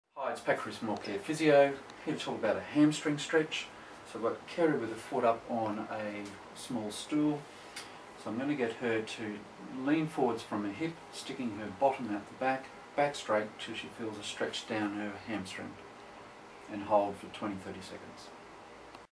All Care Physio - Knee Exercises - Hamstring Stretch
All Care Physiotherapy uses the hamstring stretch for stretching tight muscles at the back of the thigh and leg.